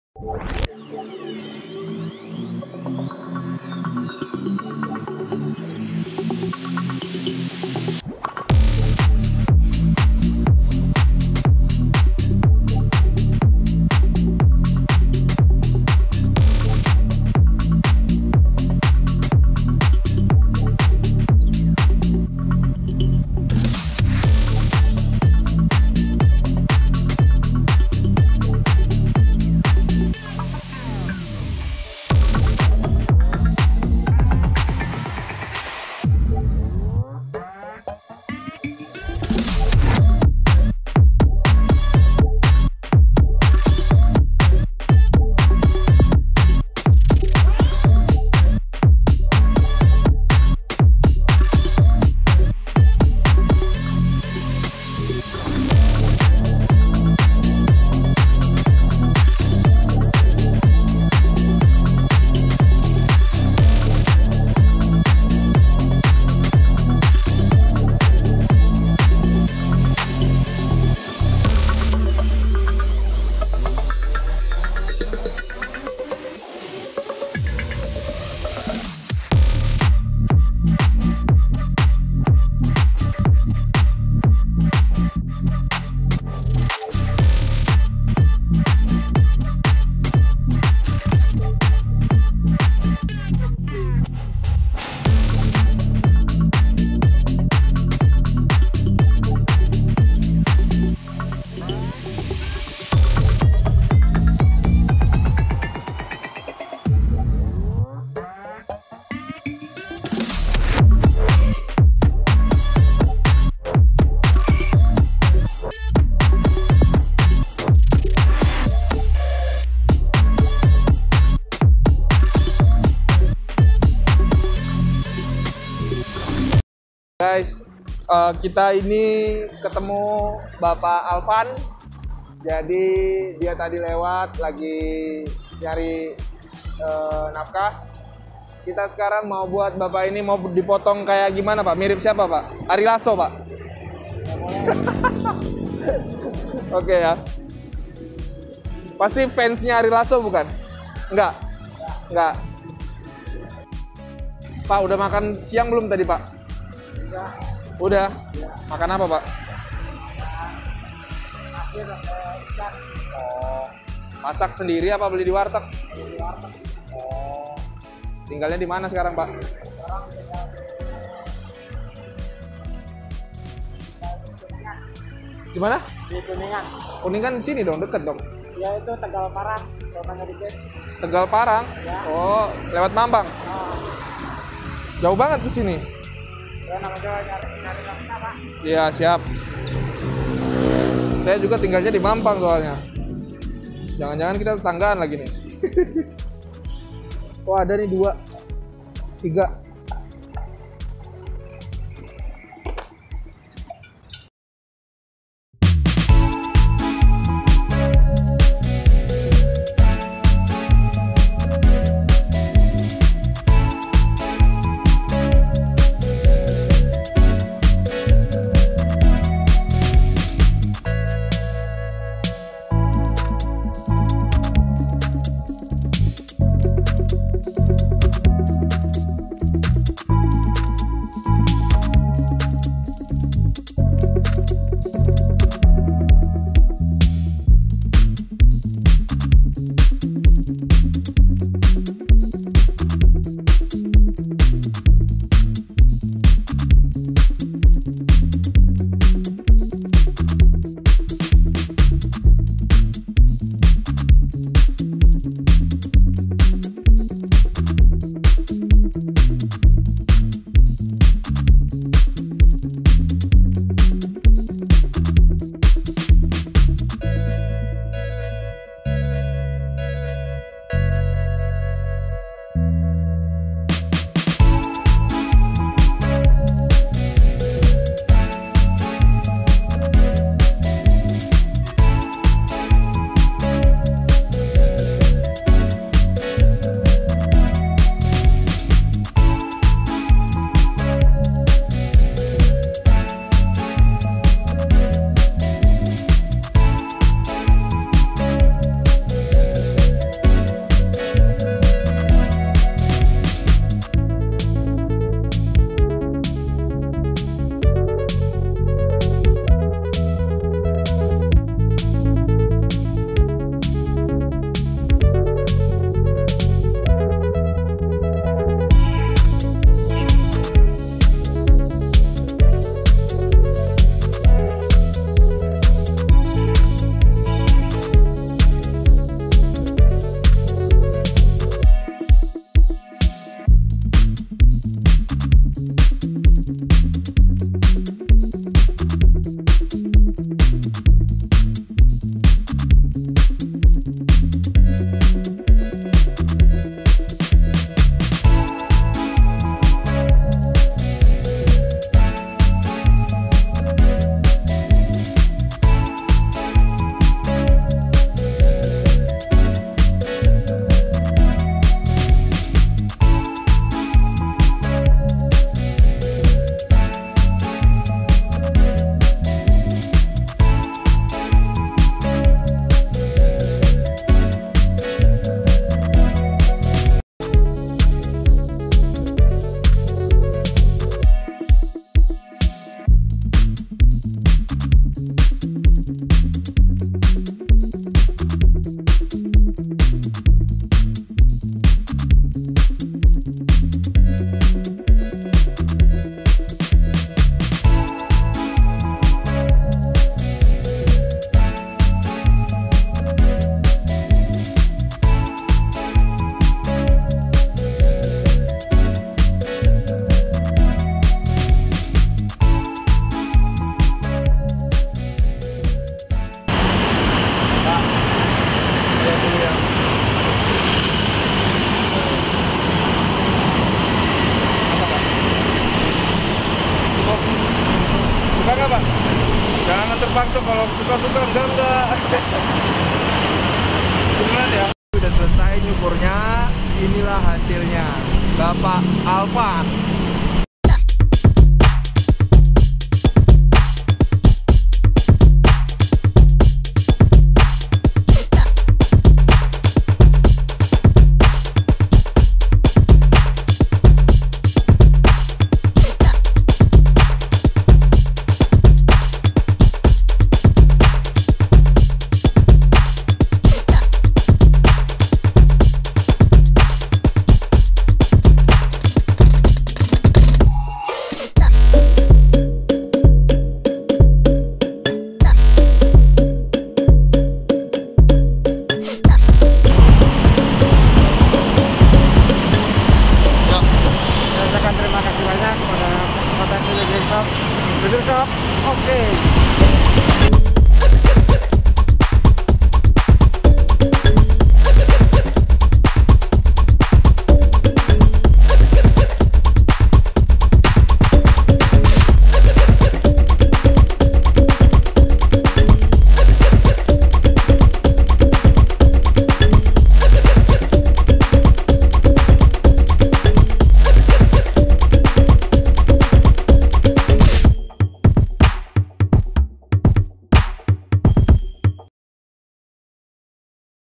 Razia Rambut Berantakan, Fantastis Barbershop
hai guys kali ini kita merapikan rambut teman teman kita di jalanan. gimna keseruannya. selamat menyaksikan.